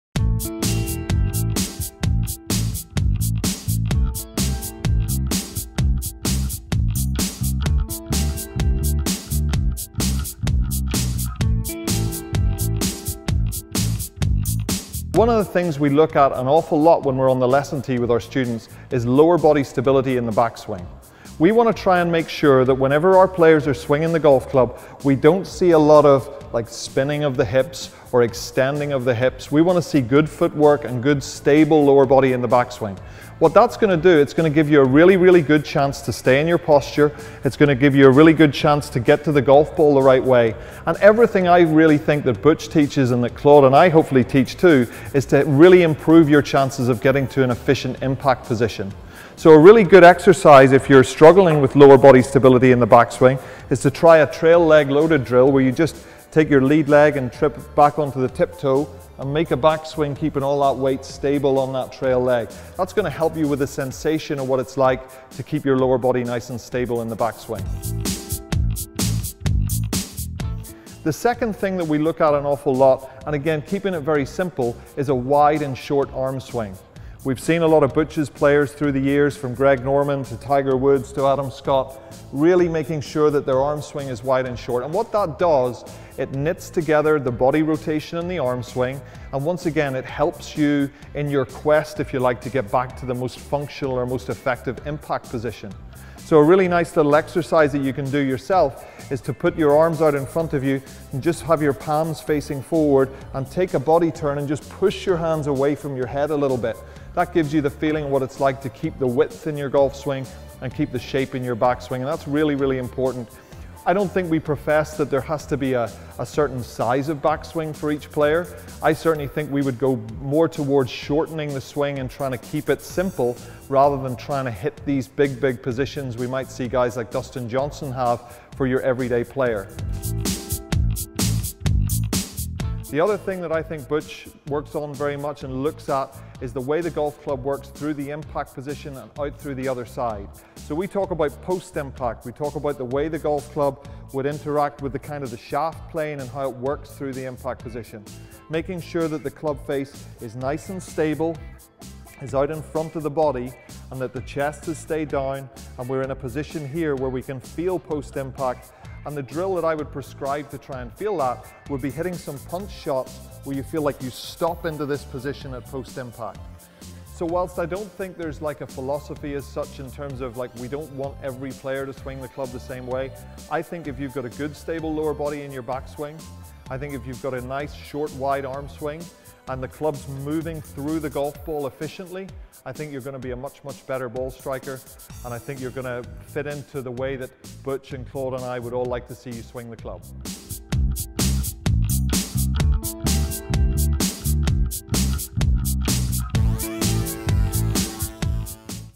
What is Butch Harmon's golf swing philosophy?
He’s the world’s leading golf coach, but what is Butch Harmon’s philosophy on the swing? In this video, Butch Harmon School of Golf director instruction, Justin Parsons, details three of his core principles with drills for each. Follow these key Harmon principles to build a more powerful, efficient golf swing